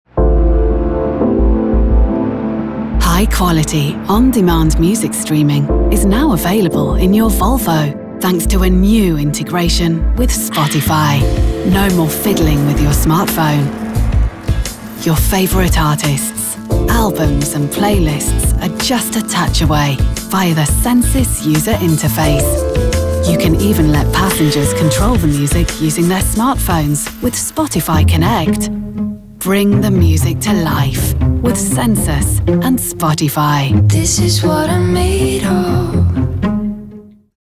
Vive la música con Sensus y Spotify
Volvo Cars integrará el servicio de Spotify en todos los nuevos modelos.